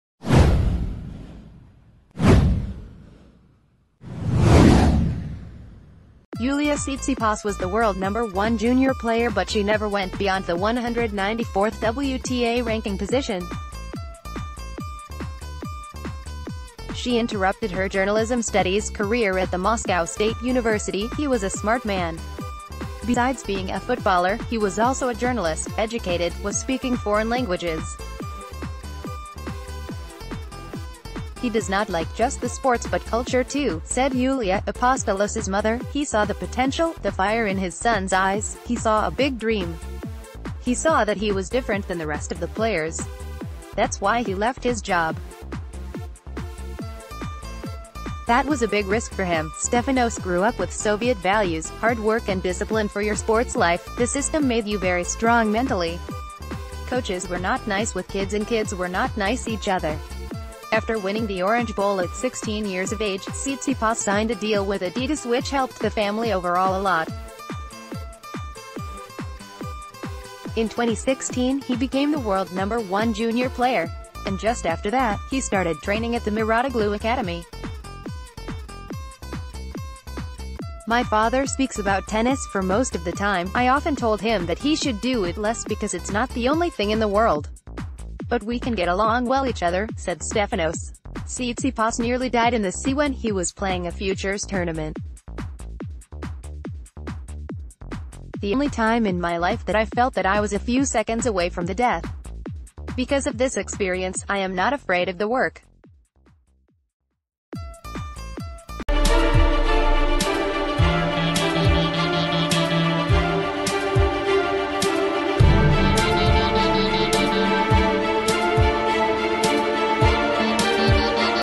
Stefanos Tsitsipas: 'My father speaks of tennis almost all the time'
Yulia Tsitsipas was the world No. 1 junior player but she never went beyond the 194th WTA ranking position. She interrupted her journalism studies career at the Moscow State University "He was a smart man. Besides being a footballer, he was also a journalist, educated, was speaking foreign languages. He does not like just the sports but culture too", said Yulia, Apostolos's mother."He saw the potential, the fire in his son's eyes, he saw a big dream. He saw that he was different than the rest of...